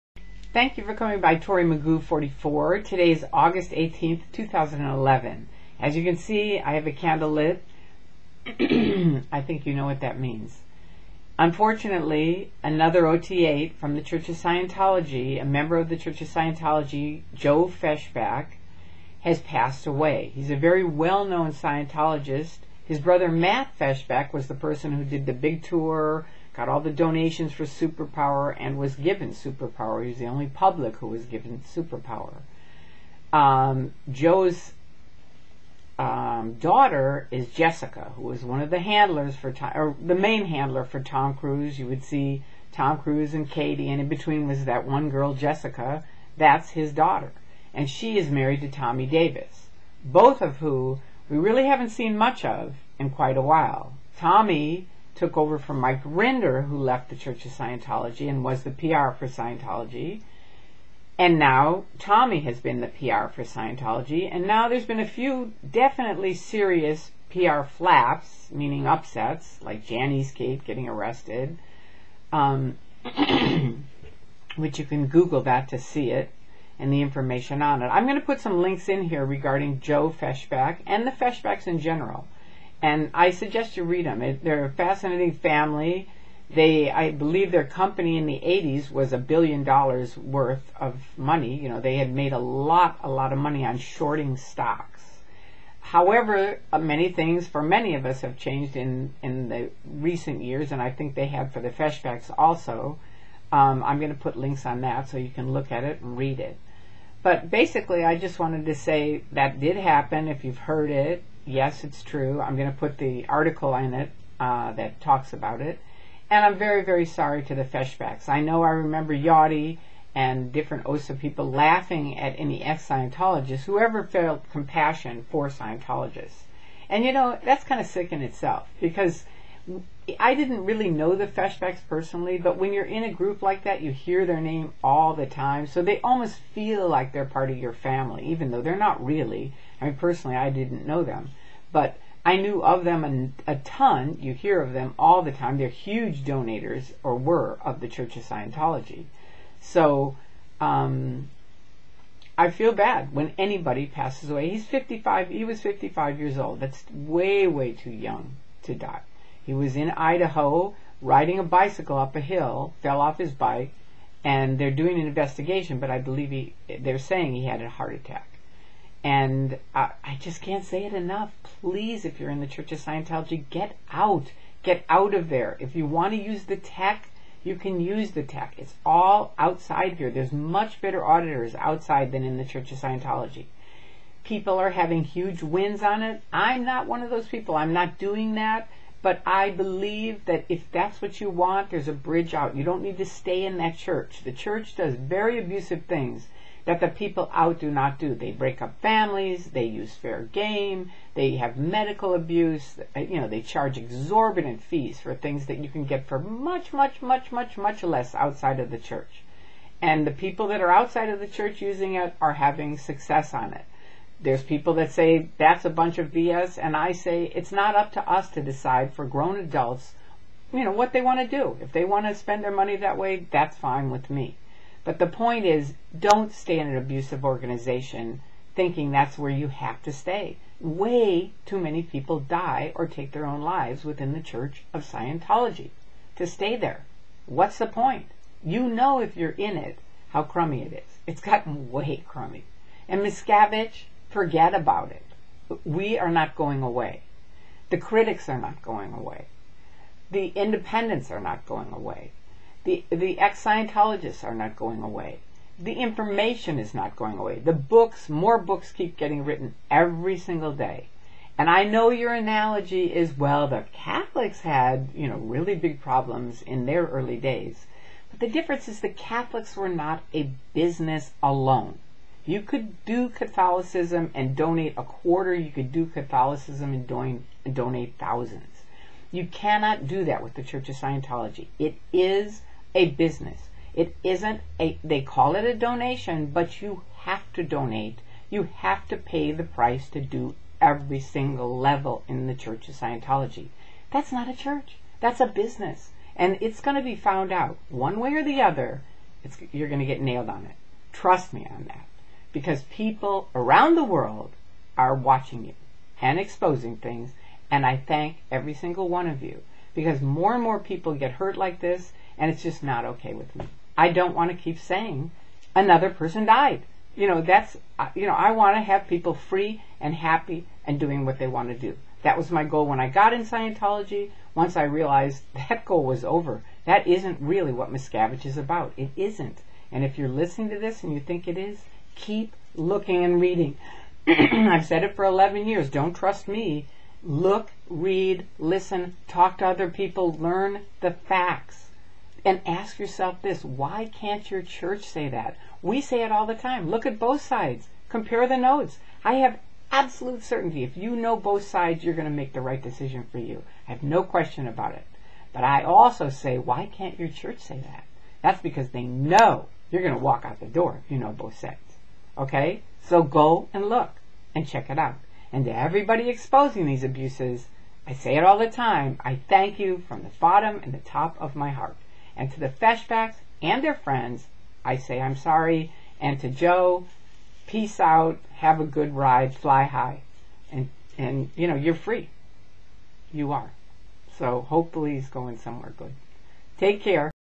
Scientology OT 8 Joe Feshbach Dead at 55
One more member of the "church" of $cientology died, Friday, at the age of 55.
Here are some links about Joe Feshbach and his family. Please take some time to read the links, and if you're "in": BAIL WHILE YOU STILL CAN!~
Please light a candle for Joe to help light his way.
A song for Joe and friends over the rainbow...